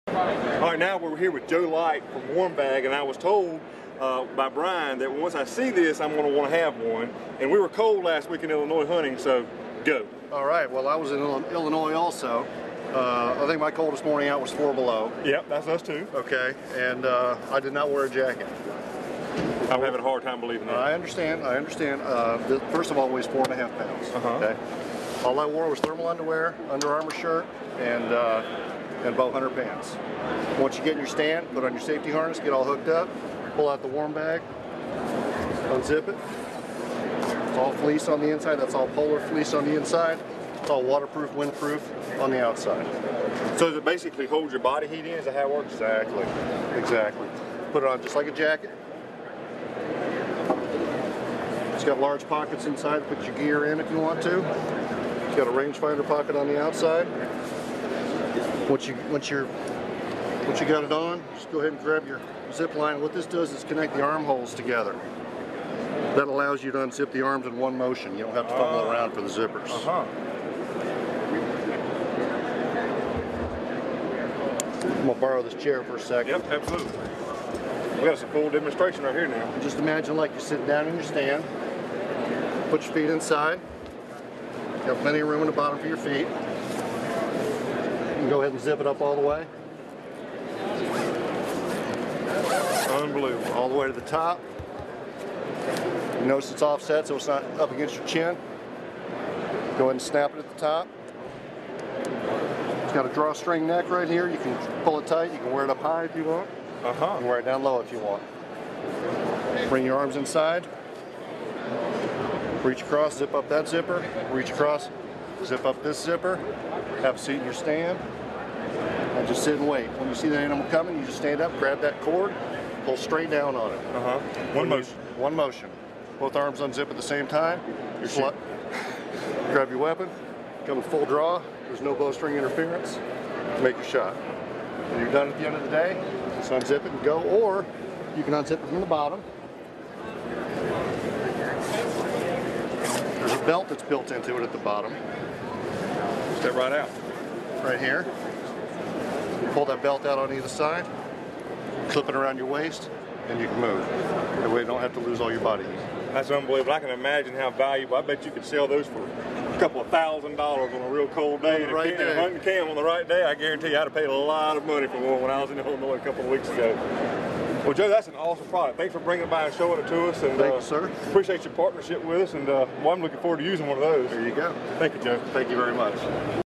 The Warm Bag at the SHOT Show
Realtree checks out the new Warm Bag at the 2010 SHOT Show.